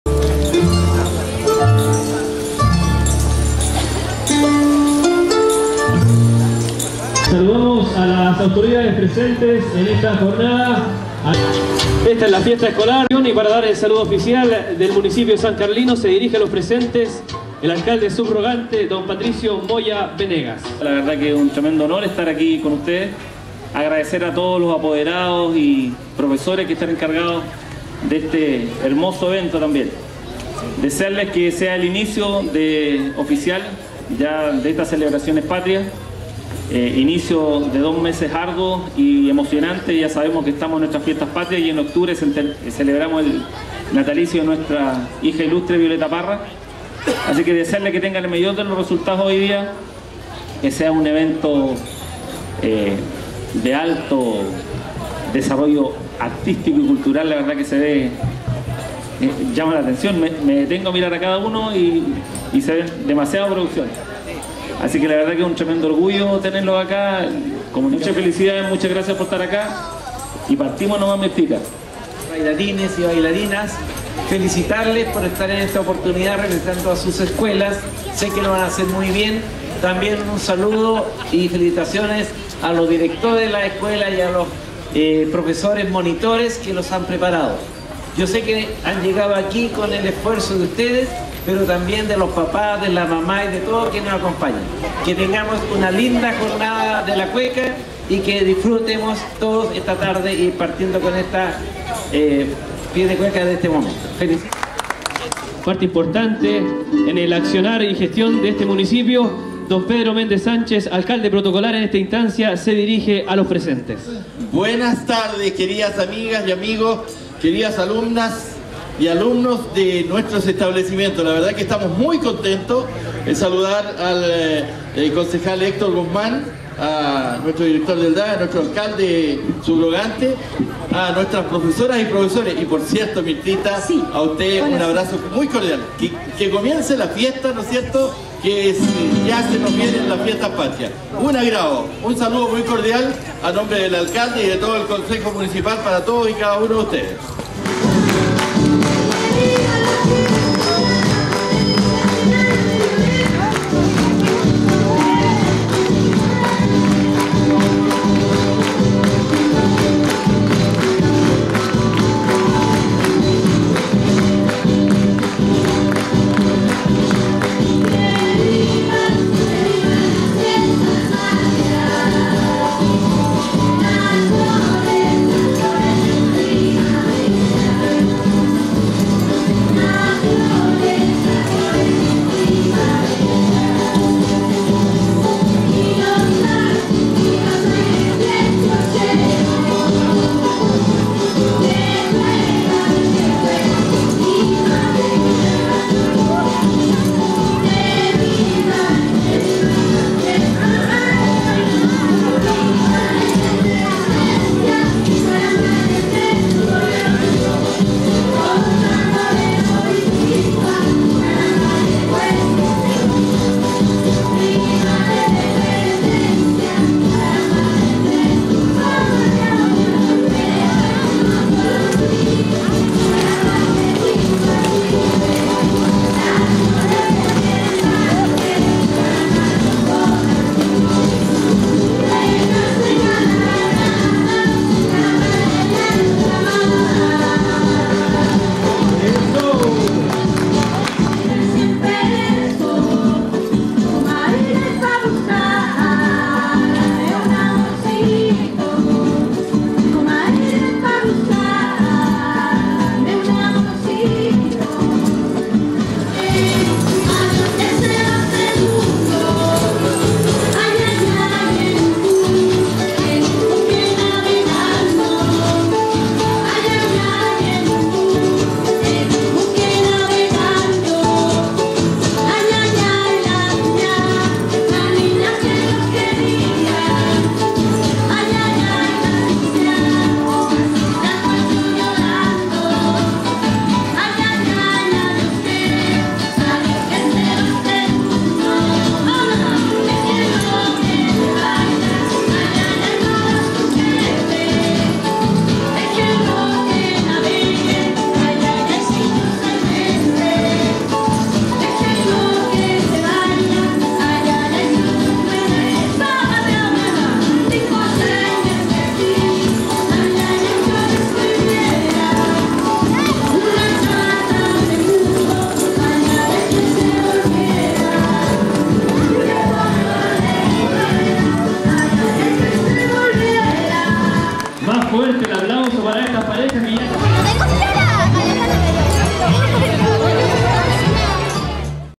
SALUDO FOLCLORICO
Saludo folclórico a las autoridades de la comuna con ocasión del comunal de cueca escolar San Carlos 2017